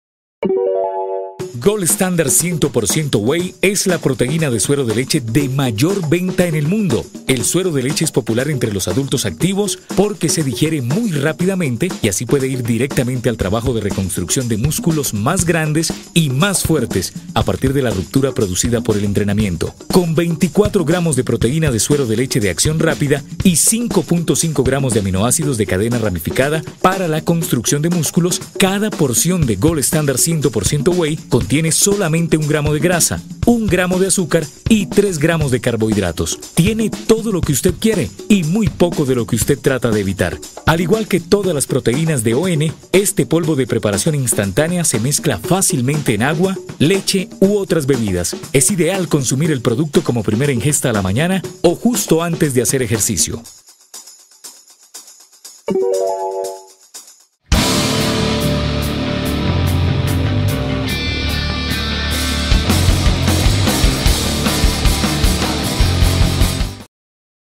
Proteína ON - Gold Standar - Whey Gold ON
- Gold Standar - Whey Gold ON - Whey Gold Standar. Gold Standar 100% Whey es la proteína de suero de leche de mayor venta en el mundo. Contiene 24 grs. de proteína de suero de leche y 5.5 grs. de aminoácidos de cadena ramificada y solo 1 g de grasa, 1 g de azúcar y 3 grs. de carbohidratos.